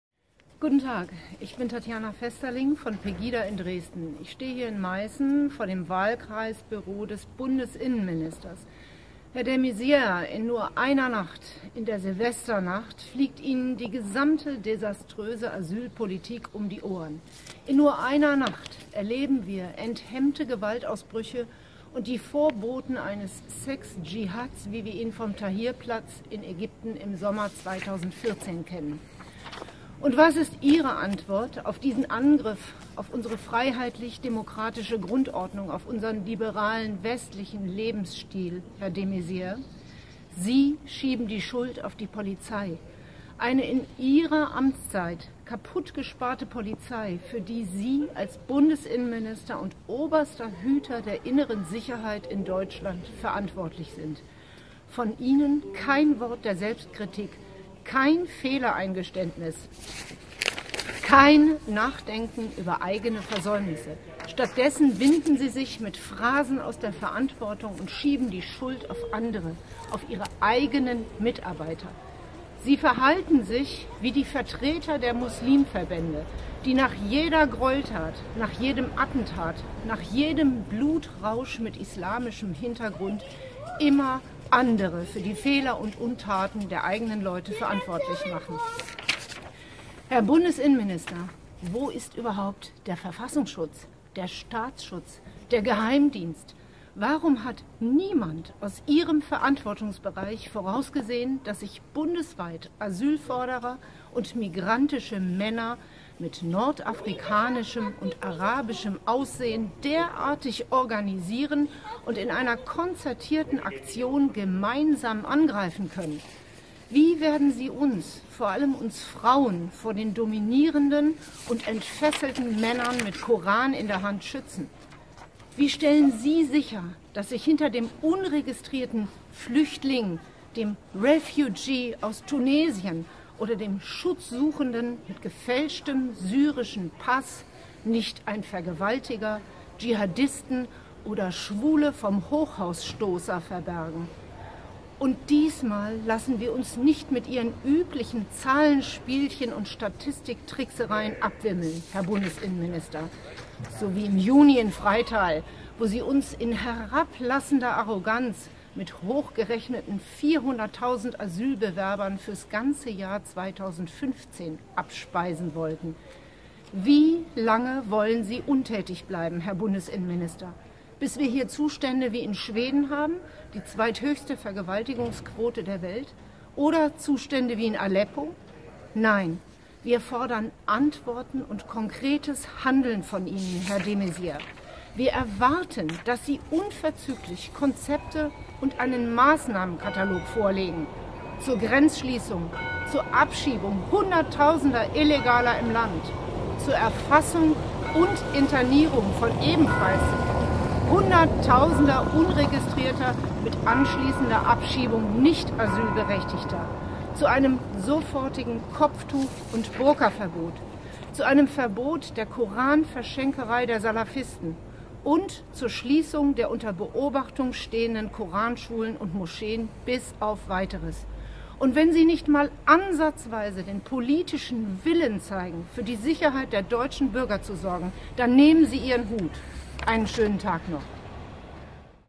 Fragen und Forderungen an den Bundesinnenminister Thomas de Maizière
Questions and claims on the Federal Interior Minister Thomas de Maizière

Außer unverschämten Schuldzuweisungen an die Polizei und die Mahnung an die Bevölkerung, bloß keinen Generalverdacht gegenüber Flüchtlingen auszusprechen,
kommt vom Minister bisher nichts.
Ja nicht mal ein Wort der Selbstkritik, ein Fehlereingeständnis oder ein Nachdenken über eigene Versäumnisse der von ihm unterstützten "Wir-schaffen-das"-Asylpolitik.

De Maizière ist oberster Hüter der innerdeutschen Sicherheit
- wo war der Verfassungsschutz? Wo der Staatsschutz?
Der Geheimdienst?

Warum hat niemand aus de Maizière's Verantwortungsbereich gesehen, dass sich bundesweit Asyl-Forderer und migrantische Männer “mit nordafrikanischem und arabischem Aussehen” derartig organisieren und in einer konzertierten Aktion gemeinsam angreifen können?

Wie will dieser Minister uns Bürger, vor allem uns Frauen, vor den entfesselten Männern mit Koran in der Hand schützen?

Wie stellt er sicher, dass sich hinter dem unregistrierten “Flüchtling”, dem “Refugee” aus Tunesien oder dem “Schutz-Suchenden” mit gefälschtem, syrischen Pass - nicht Vergewaltiger, Djihadisten oder Schwule-vom-Hochhaus-Stoßer verbergen?

Und wir lassen den Minister diesmal nicht mit den sonst üblichen Zahlen-Spielchen und Statistik-Tricksereien davon kommen
- wir fordern Antworten und konkretes Handeln von Ihnen,
Herr de Maizière!

Wir erwarten unverzüglich Konzepte und das Vorlegen eines Maßnahmenkataloges, zu dem selbstverständlich das Aufstocken der Polizei gehört.

Weiter erwarten wir Antworten und Lösungen zu folgenden Forderungen:

-    zur Grenzschließung
-    zur Abschiebung Hundertausender Illegaler im Land
-    zur Erfassung und Internierung von ebenfalls Hunderttausender  Unregistrierter mit anschließender Abschiebung nicht Asylberechtigter
-    zu einem sofortigen Kopftuch- und Burkaverbot
-    zu einem Verbot der Koran-Verschenkerei der Salafisten
-    und zur Schließung der unter Beobachtung stehender Koranschulen und Moscheen bis auf Weiteres.

Und wenn der Bundesinnenminister weiterhin keinen politischen Willen zeigen, für die Sicherheit der deutschen Bürger zu sorgen, dann soll er abtreten - sofort!

After the nationwide attacks on New Years Eve:
Questions and claims
on the Federal Interior Minister Thomas de Maizière

Besides outrageous accusations to the police and the exhortation to the people, to express no general suspicion towards refugees, from this Minister comes nothing so far.
Not even a word of self-criticism, a fault confession or thinking about their own failings of this by him supported "We-achieve-that" asylum politics.

De Maizière is the supreme guardian of the German internal security - which was the protection of the constitution?  Where the state protection? The secret service?

Why has nobody seen from the Maizière's area of responsibility that asylum seekers and immigrant men "with North African and Arab appearance" can organize an attack together in such a concerted effort nationwide?

How will this minister protect us citizens, especially us women, from the raging men with Koran in their hands?

How will he assure that behind the unregistered "refugees", the "Refugee" from Tunisia or the "protection-seekers" with fake,
syrian passports - hide no rapists, jihadists or gays-of-the-skyscraper-pushers?

And this time we will not allow the minister to come with the usual shenanigans numbers and statistical shenanigans - we demand answers and action from you, Mr. de Maizière!

We expect immediate concepts and presenting a catalog of measures, of course, including the increasing of the police. Next we expect answers and solutions to the following demands:

- The border closure
- The deportation of hundreds of thousands illegals in Germany
- To capture and internment of hundreds of thousands also unregistered, followed by deportation of all not entitled asylum seekers
- A headscarf and burqa ban immediately
- A ban on the Koran give-aways of the Salafists
- And the closure of all under observation standing madrassas and mosques until further notice.

And if the minister continues to show no political will to ensure the safety of the German citizen, then he should resign - immediately!